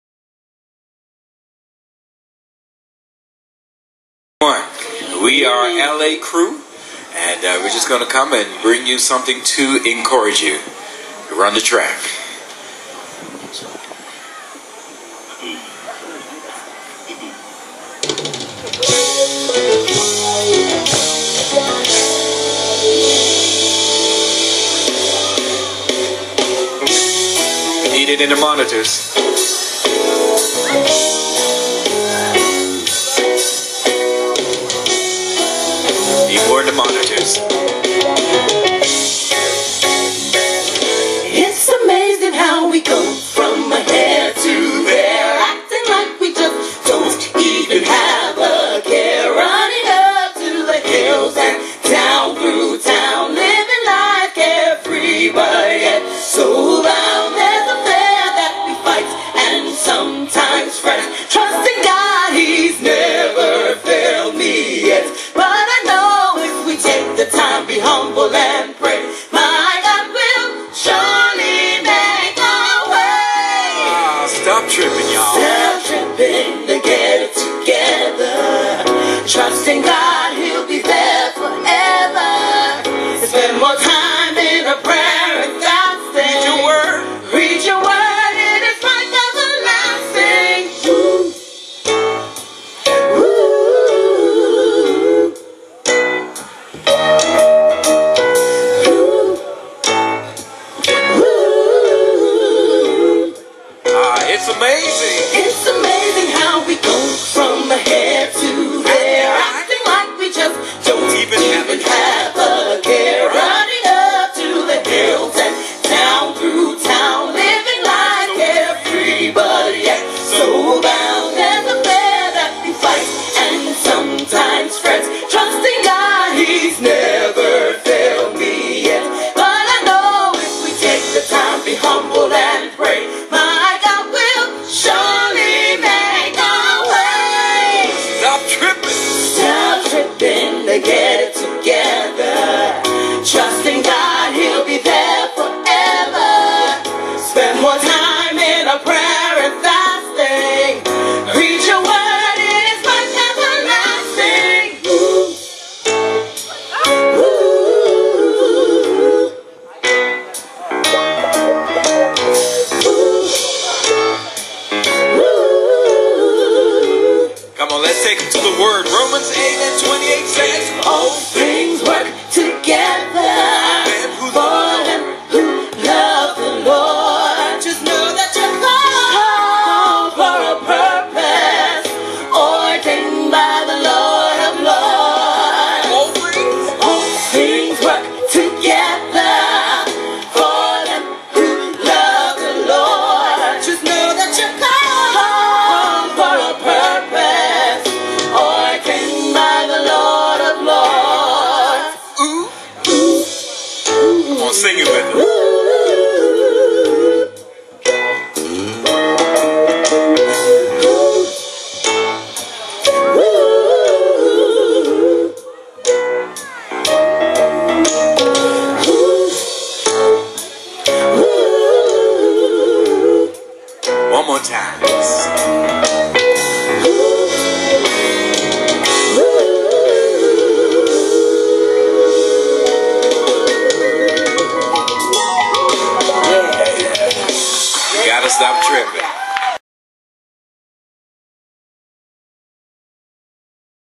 LA Crew Perform At CultureFest, Sept 29 2012
LA Crew Perform At CultureFest "Unity in the Community" — a two day celebration of all the diverse cultures in Bermuda, Sept 29 2012